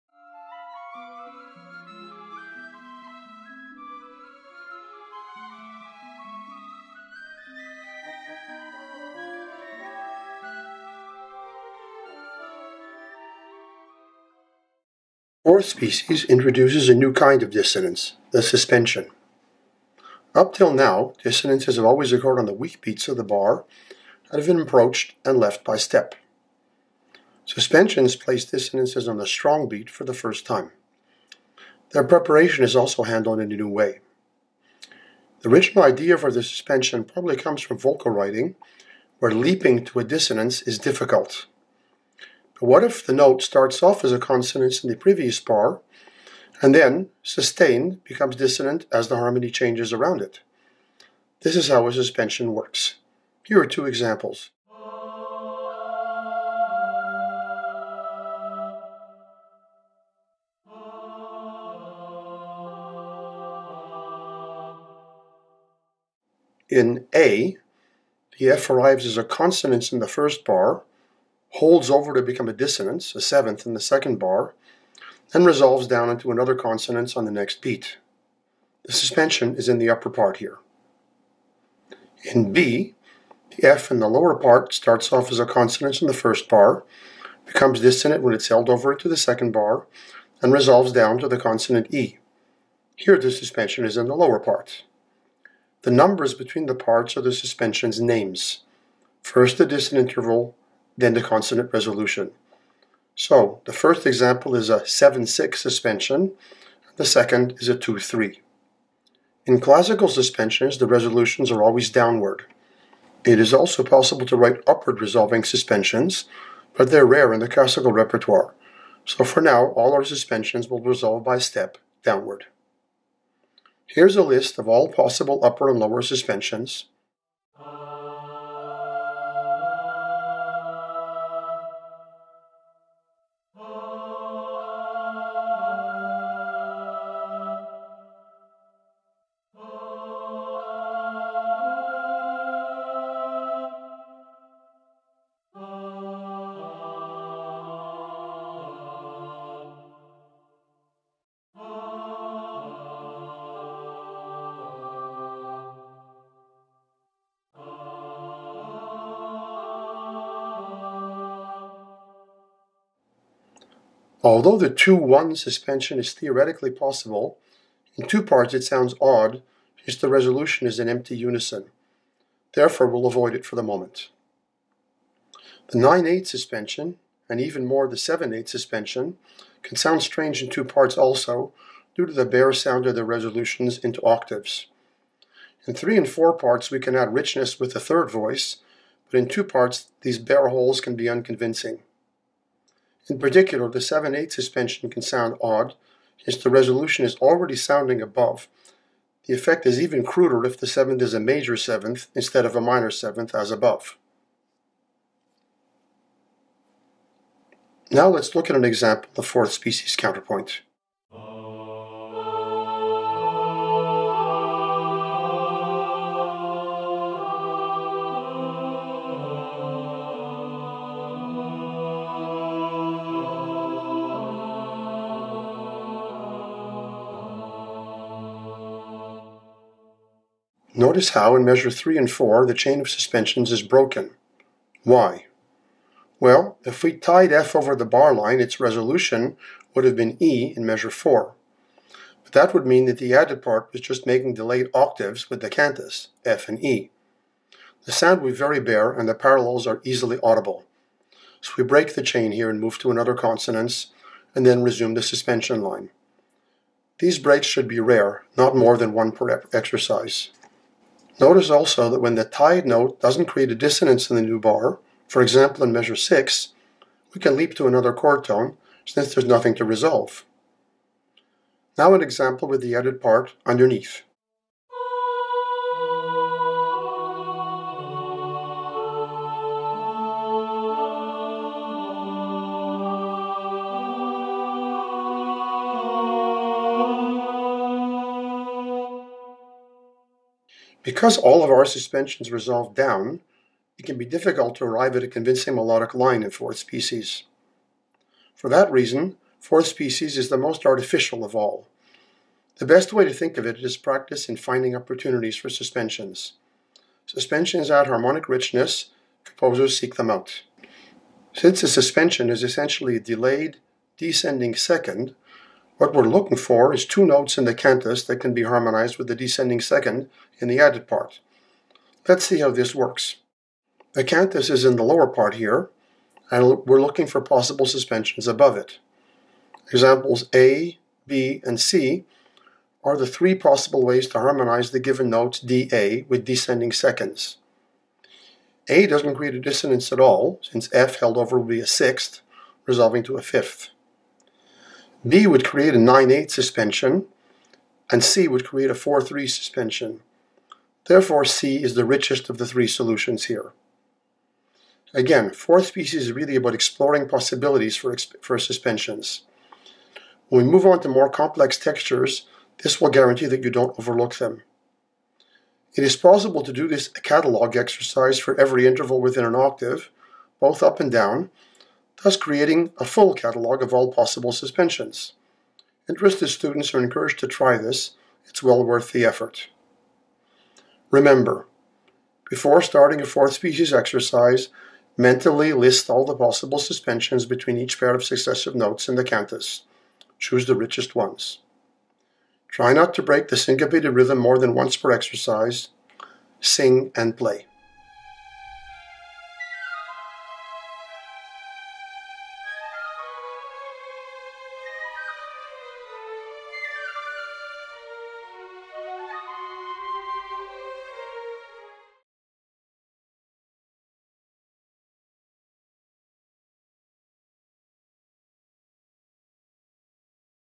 Counterpoint # 6 - Fourth species
Fourth species counterpoint (suspensions): explanation and examples.